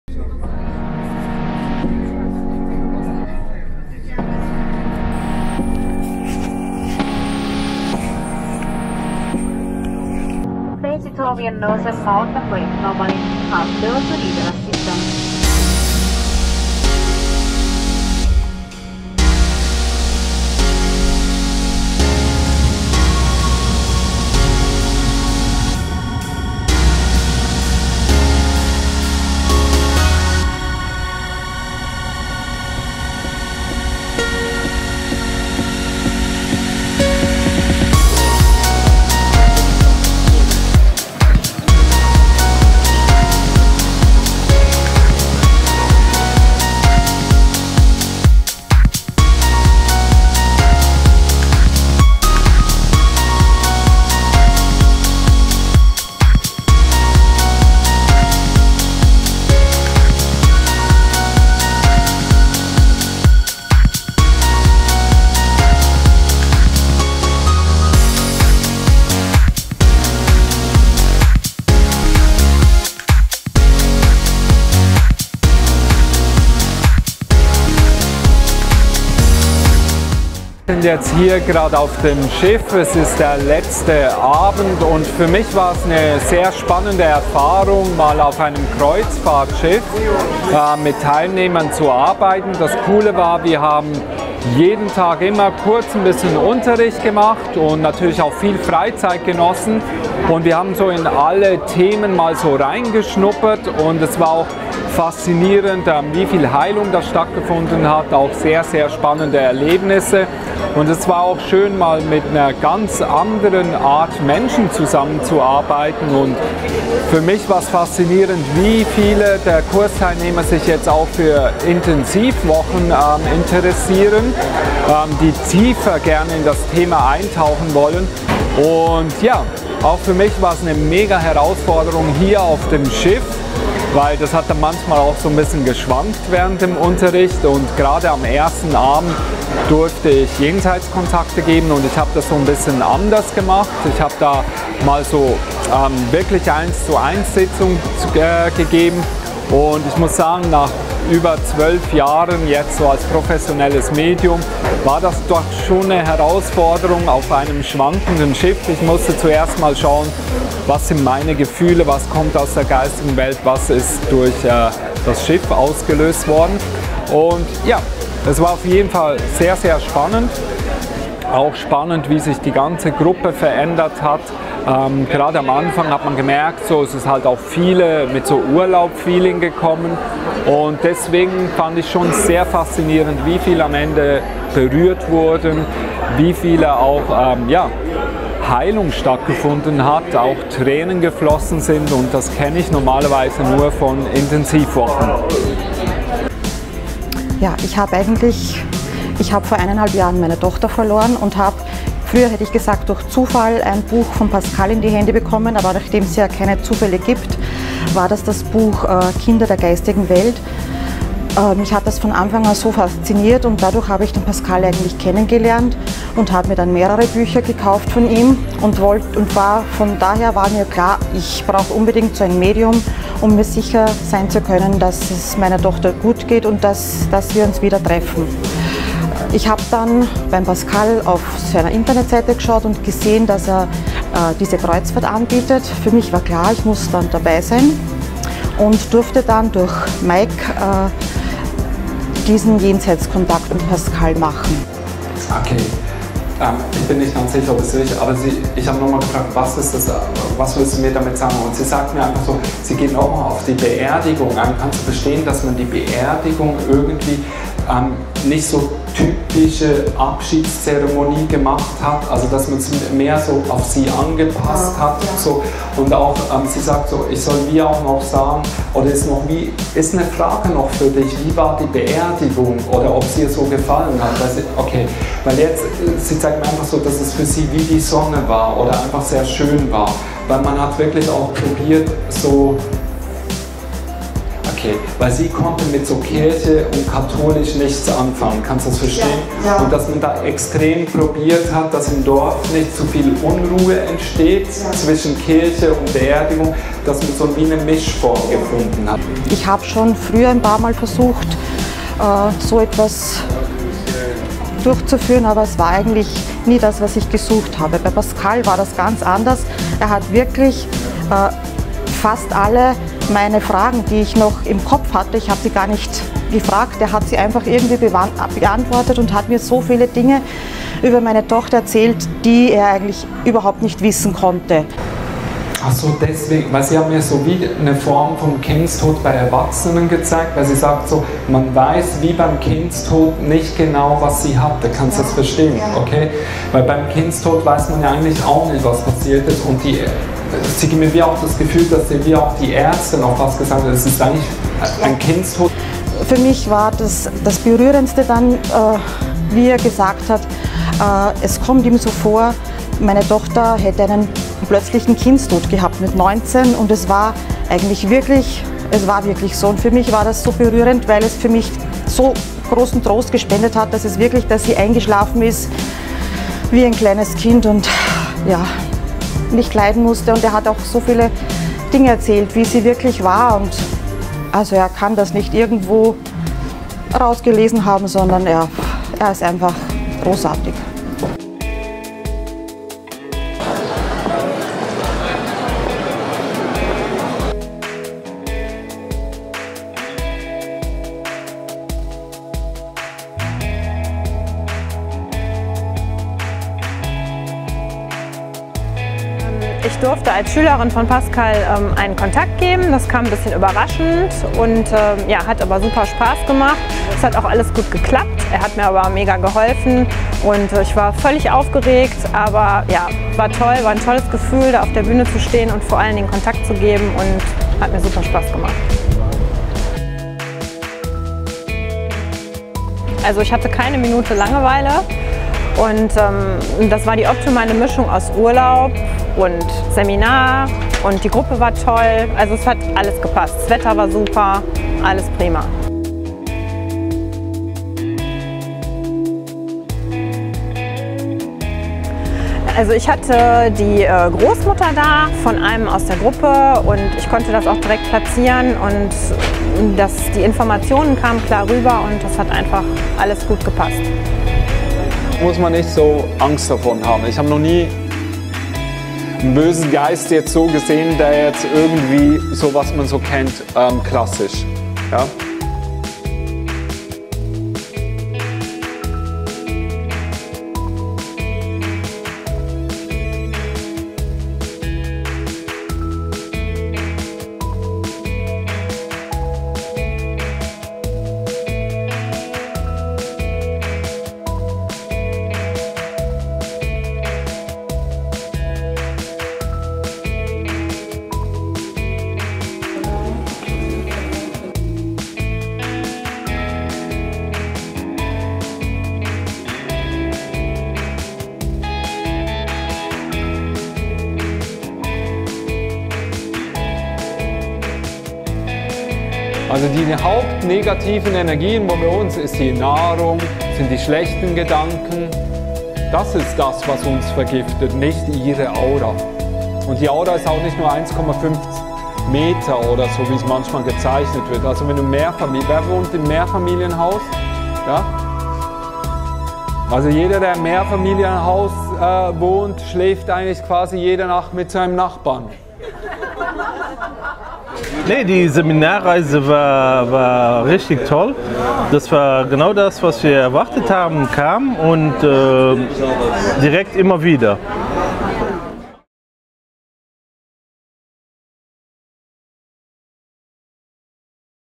TOUR VLOG#2 mit Pascal Voggenhuber - Seminarreise 2019
Pascal Voggenhuber gilt als eines der renommiertesten Medien in Europa und ist 12 facher Bestseller-Autor. Er beeindruckt und verblüfft seine Zuhörer bei Veranstaltungen immer wieder aufs Neue mit detailliertesten Jenseits-Kontakten, eindrucksvollen Demonstrationen und berührenden Vorträgen. Er erzählt über sich und seine Arbeit.